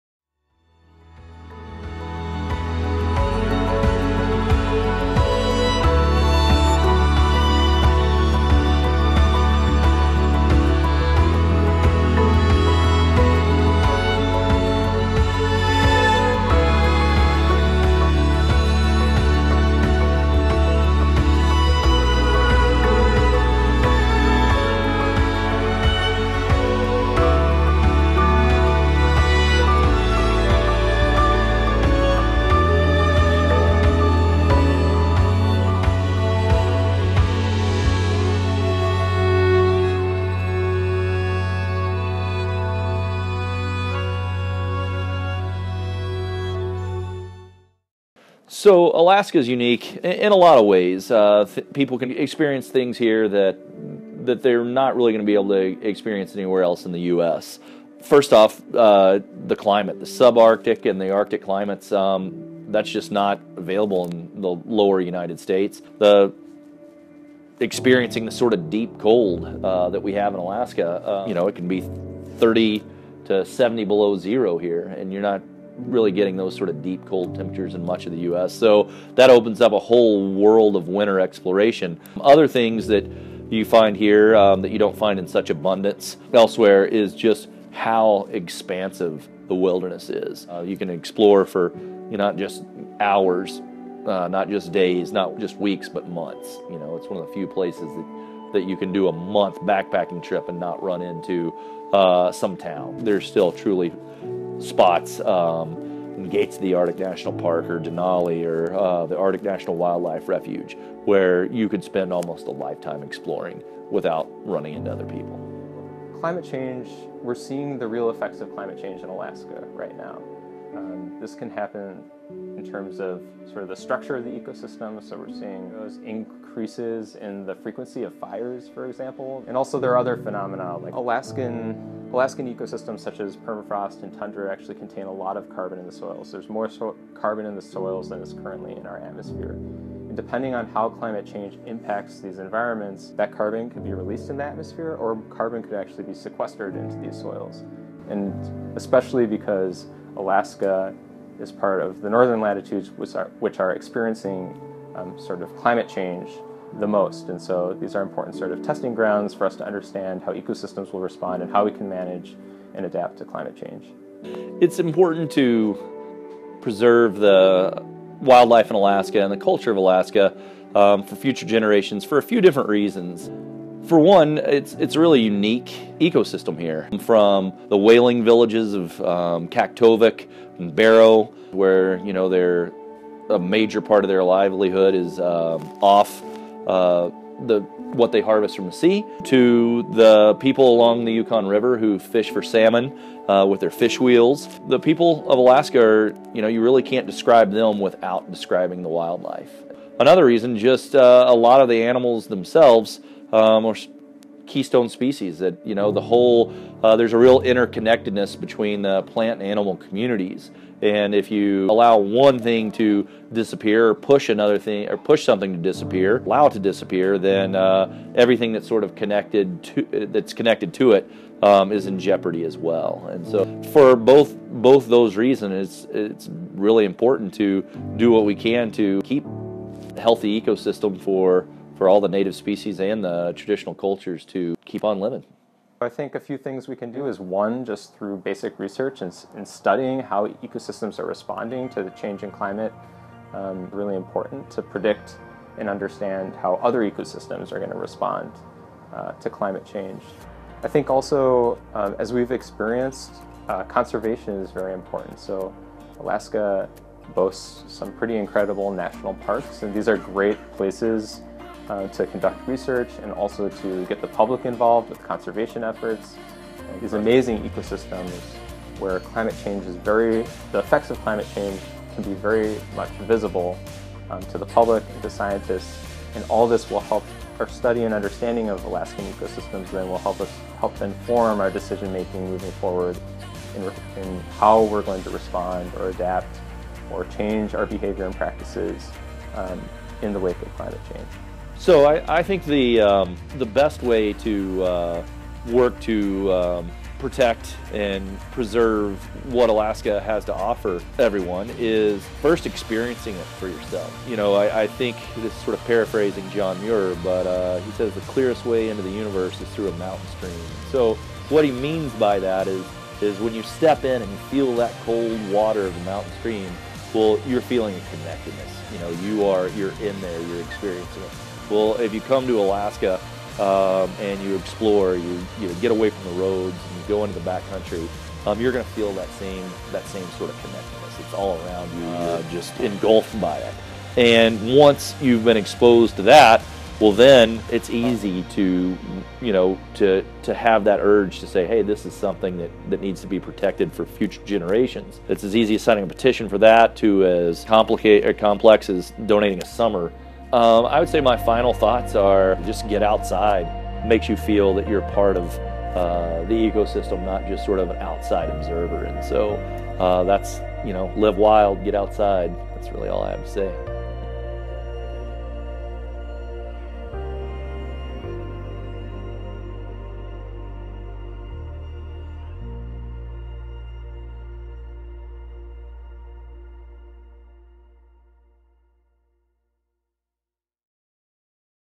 National Geographic Student Expeditions: Discovering Alaska 2015
Alaska is known for its expansive wilderness which is threatened by climate change. This short documentary focuses on why we should experience, research and preserve Alaska's unique ecosystems.  Featuring renowned National Geographic naturalist and environmentalist, Jason Matthews and National Geographic ecologist, Joey Knelman. This film was shot on a twelve day student expedition by Simon Geisker, Chris Maggio and Caleb Chew with the help of filmmaker Mike Diaz.